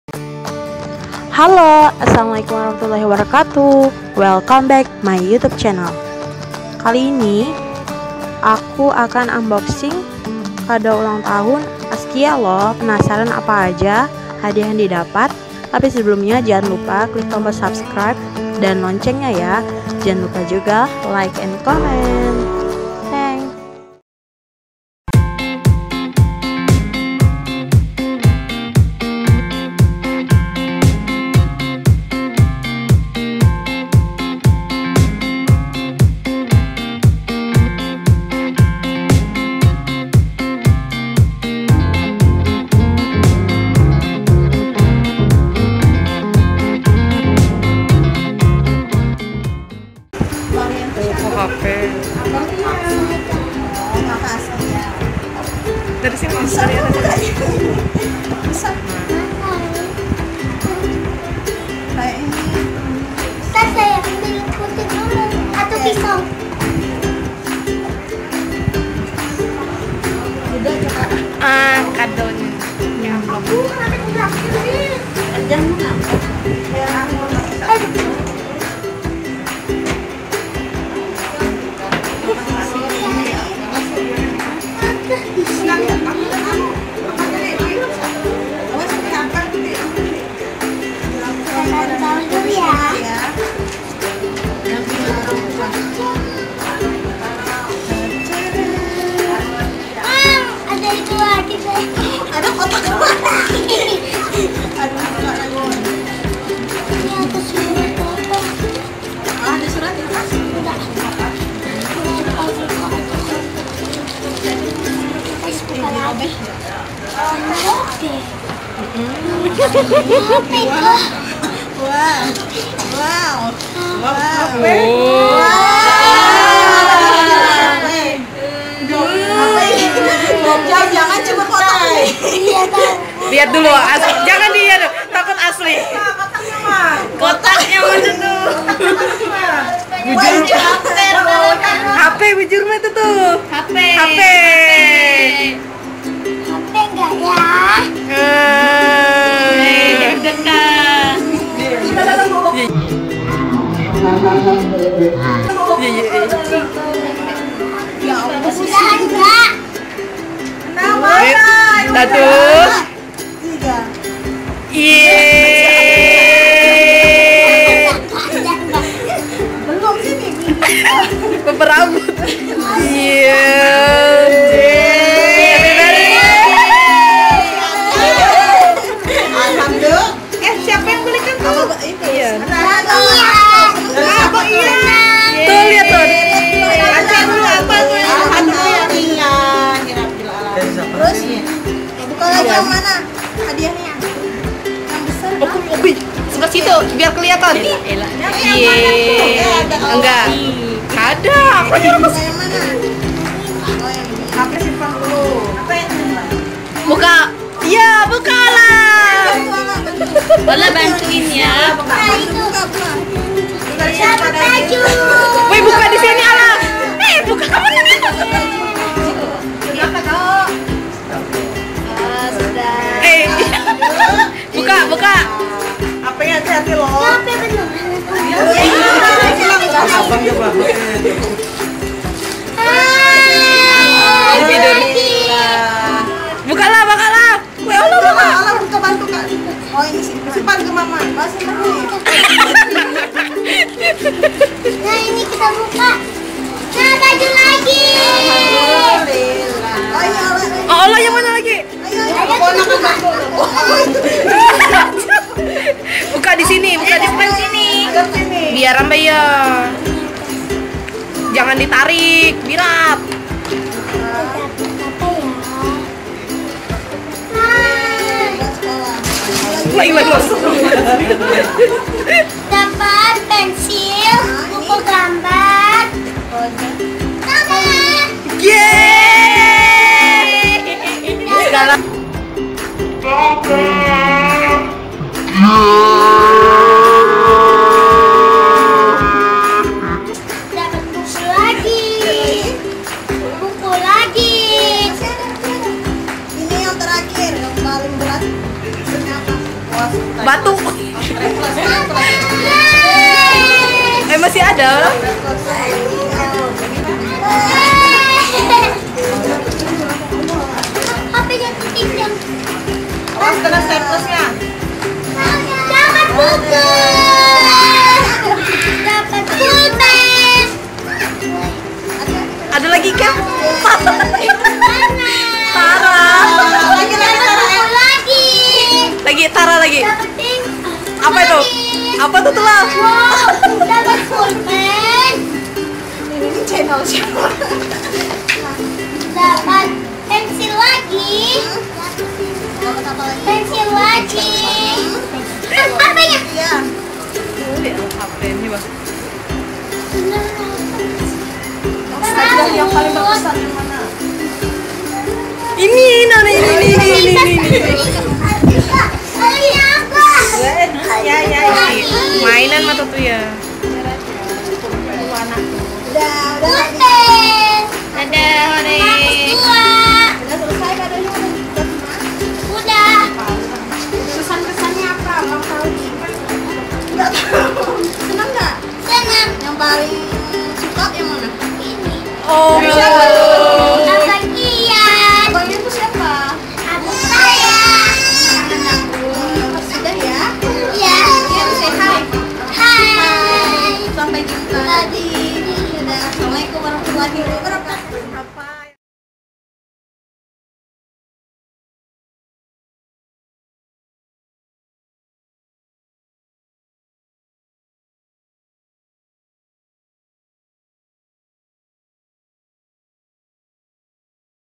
HEBOH | Unboxing kado ultah dapat hp | Siti Akobah
Heboh, saat tau di hari ultah dapat hp. Jadi kami langsung unboxing kado tersebut. Sebelumnya maaf ya guys pencahayaanya kurang bagus saat pengambilan video, soalnya dadakan😅. Unboxing kado ulang tahunya Azkia, adik ipar ku😊... dan akhirnya dia dapat barang yg sangat diingikannya yaitu hp dari hadiah ultahnya.

Kamera: Hp Samsung Galaxy Grand Prime